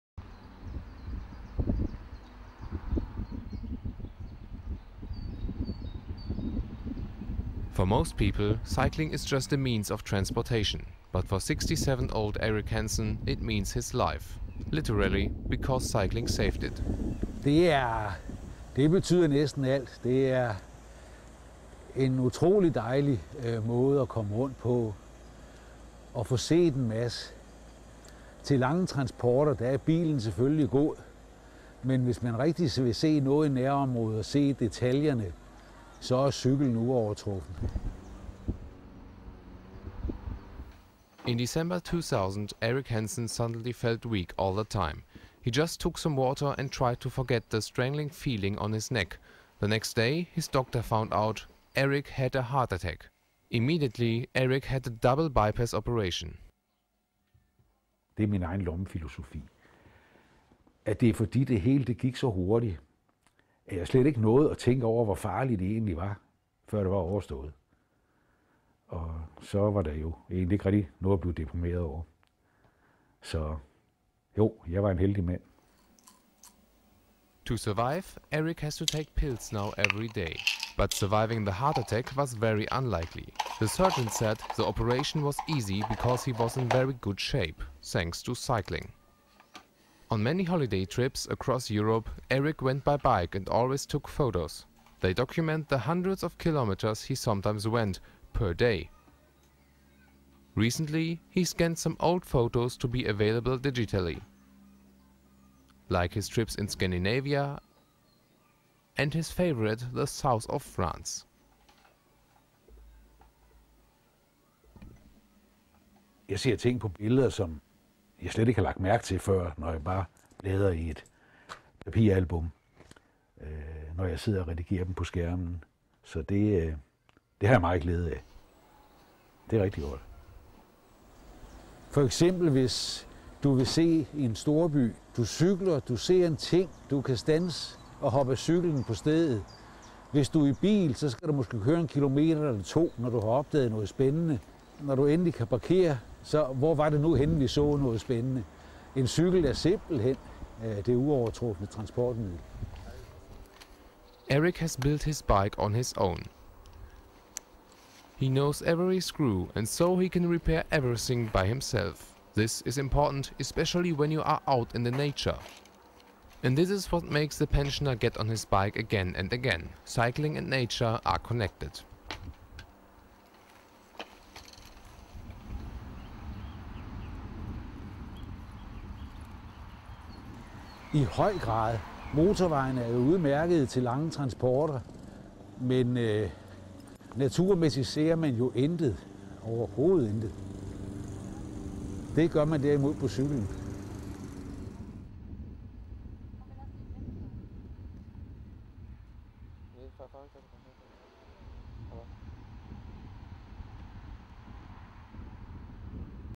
Nice cycle story of recovery after heart attack - Fahrradfahrer-Geschichte nach Herzinfarkt
One bypass operation, two artificial hips - three bicycles: Nothing stops the Dane Erik Hansen from riding a bike. He is the living proof for that cycling cannot only make you feel better  it can make a huge difference in your life.

Une pontage cardiaque, deux hanches artificielles, trois vélos: rien n'arrête le Danois Erik Hansen de rouler à vélo. Il est la preuve même que le vélo ne peut pas seulement provoquer un sentiment de bien être mais qu'il peut faire une contribution à changer votre vie.

Production:
dreamJ video solutions